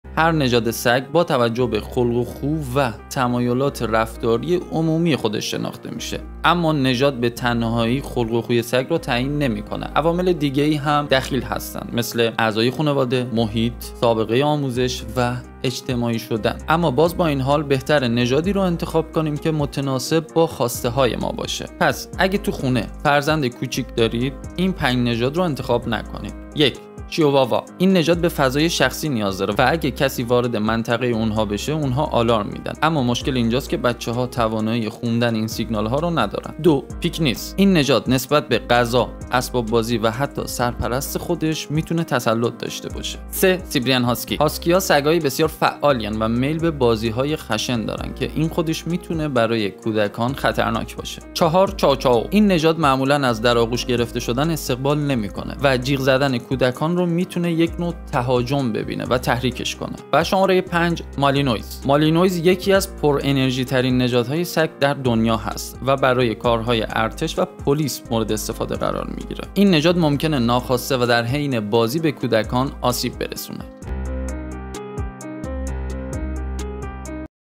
5 تا از سگ هایی که برای بودن در کنار کودکان شما خطرناکن!
دوستانی که این نژاد هارو دارین از ما ناراحت نشید
این فقط خلق و خو نژاد سگتونه.
البته نژاد نمیتونه به تنهایی نمیتونه تاثیر زیادی داشته باشه و بخش زیادی برمیگرده به تربیت درست و اصولی سگ !
در آینده نژاد های بیشتری رو بهتون معرفی می‌کنیم ...